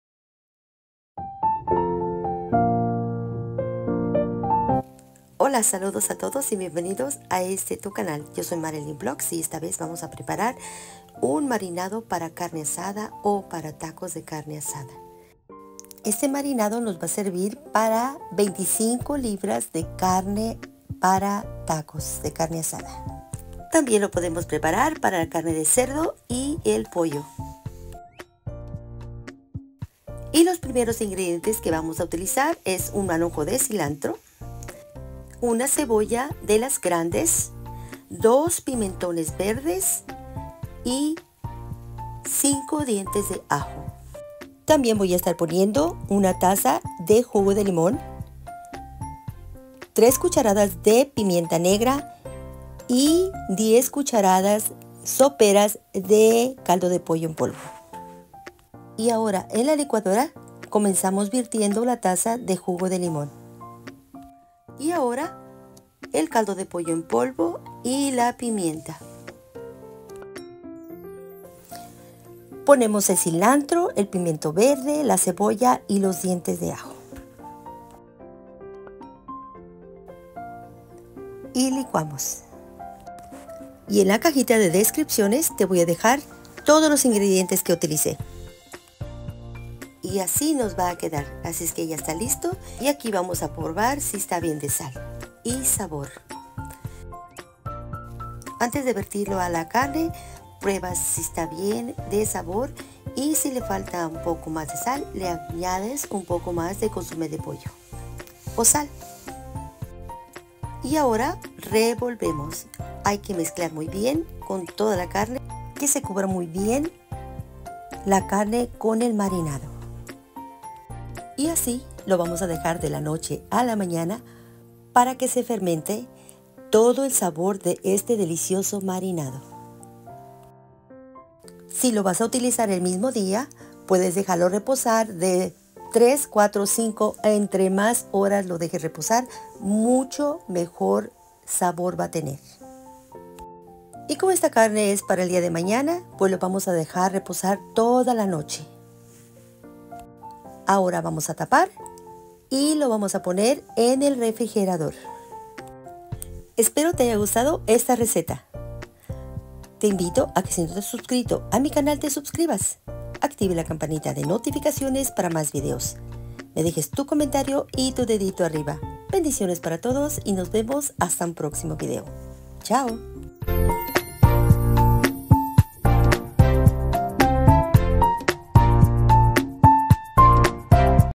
DELICIOSO MARINADO PARA CARNE ASADA
MARINADO PARA CARNE ASADA
Para esta receta necesite los siguientes …..

INGREDIENTES:
Carne de Res 23-25 lbs
Cebolla 🧅
Ajos 5 dientes
Cilantros 1 manojo
Chile Pimentón 2
Jugo de Limon 1 Taza
Pimienta Negra 2-3 Cucharadae
Consome de pollo 8-10 Cucharadas